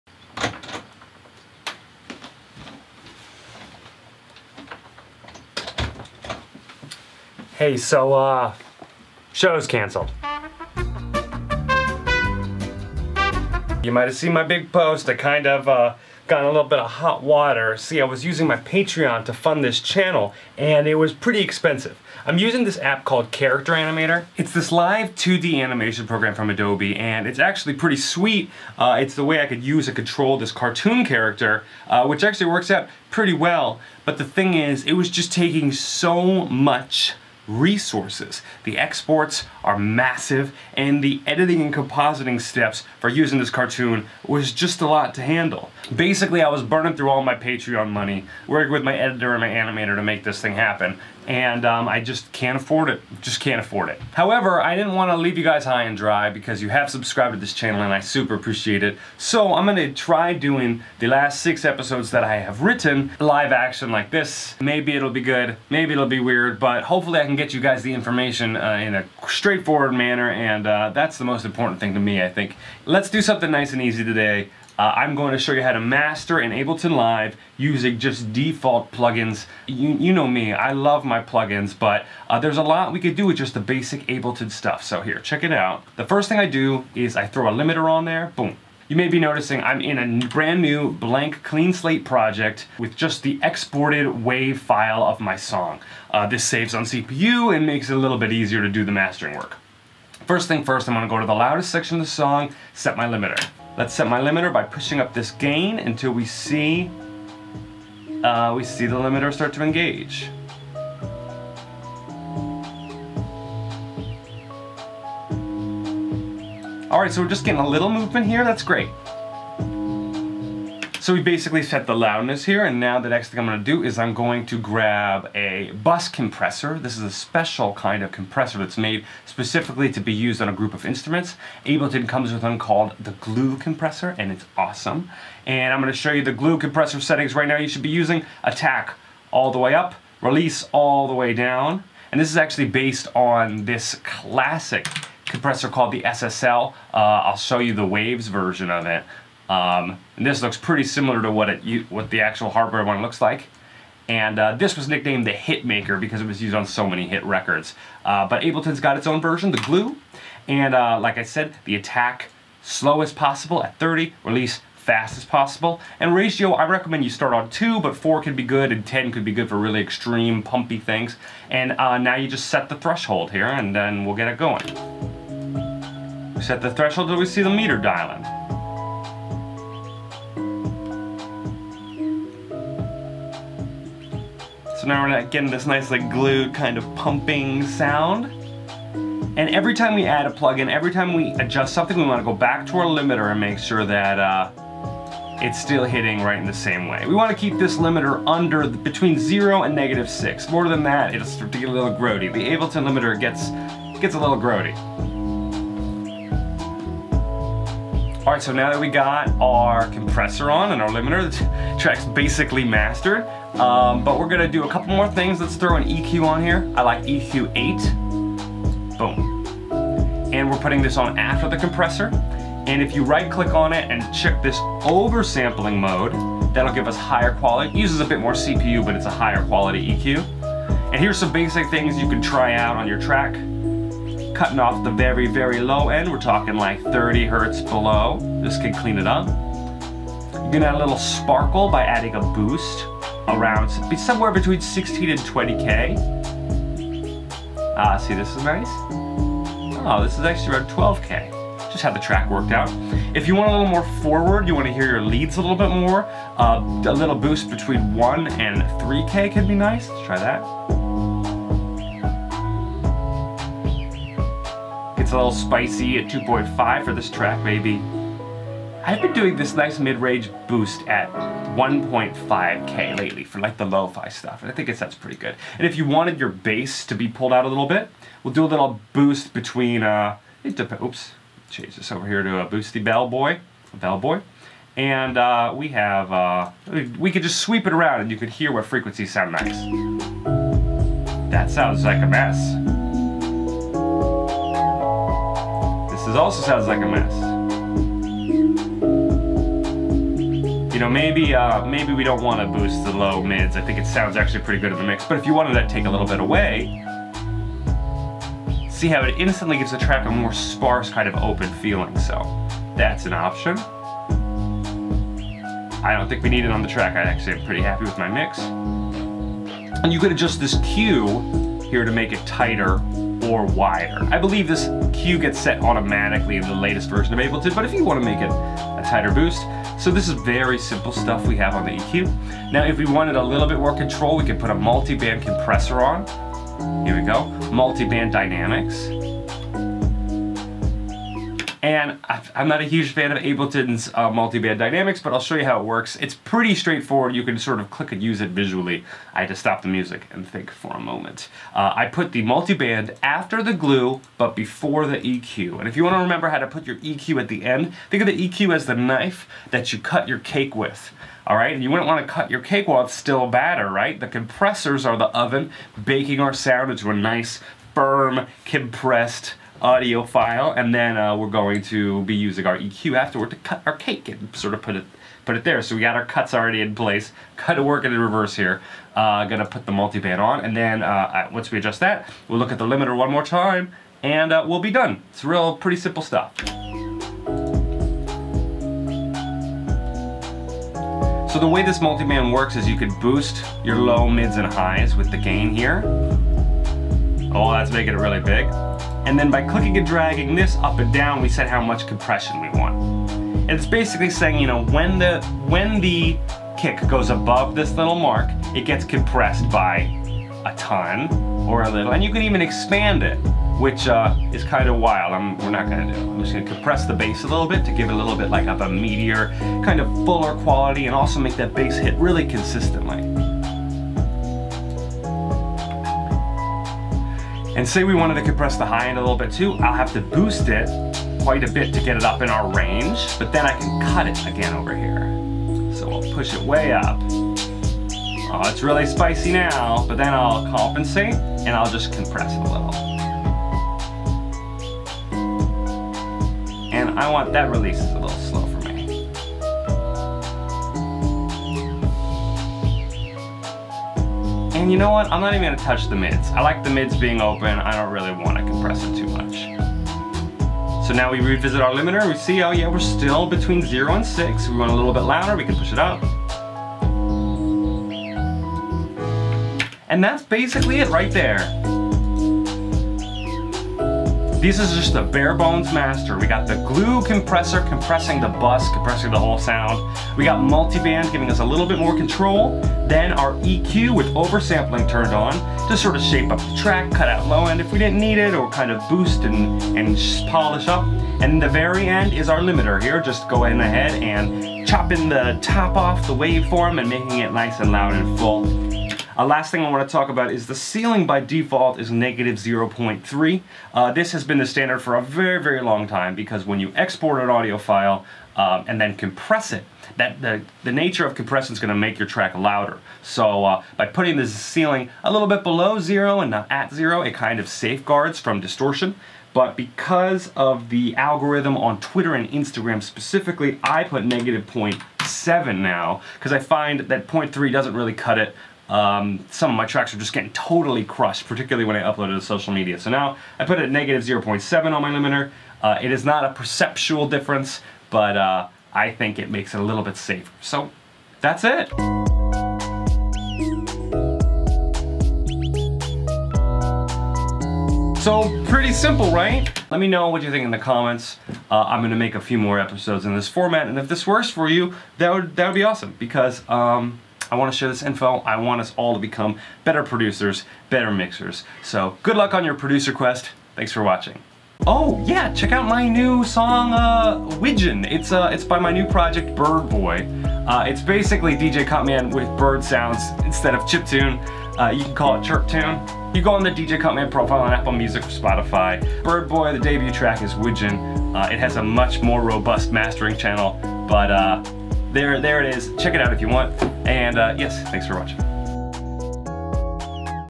Mastering in Ableton Live without Plug-Ins
Learn how to master using Ableton Live 10 with just it's stock effects, the things that come free with the program. Even without extra plug-ins, we do a great basic mastering job to any piece of music or audio using Ableton's built in Equalizer, Compressor, Multiband and Limiter.

Any questions? Ask me in the comments!